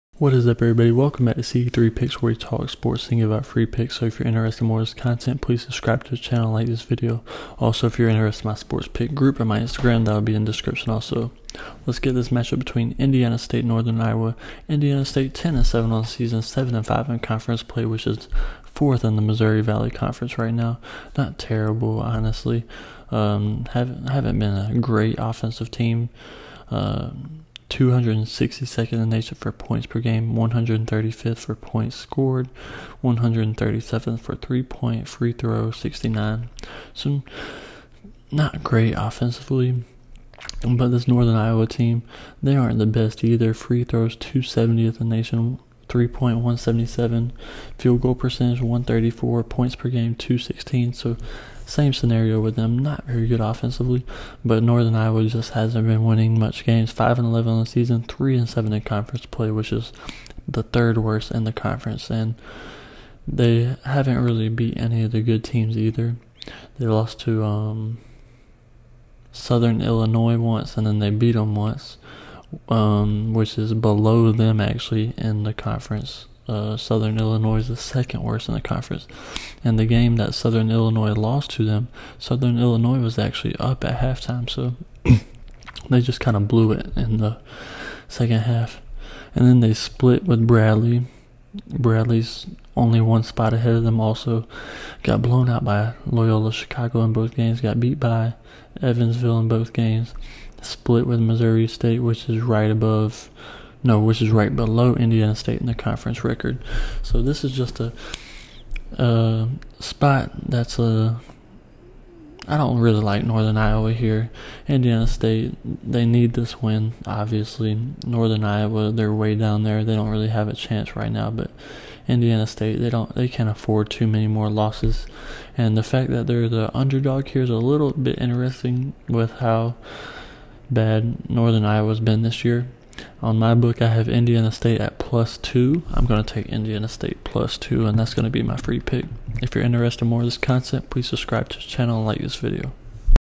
Indiana St. vs Northern Iowa 2/6/21 Free college basketball pick, tip, and projection.
The Indiana State Sycamores take on the Northern Iowa Panthers on February 6th, 2021.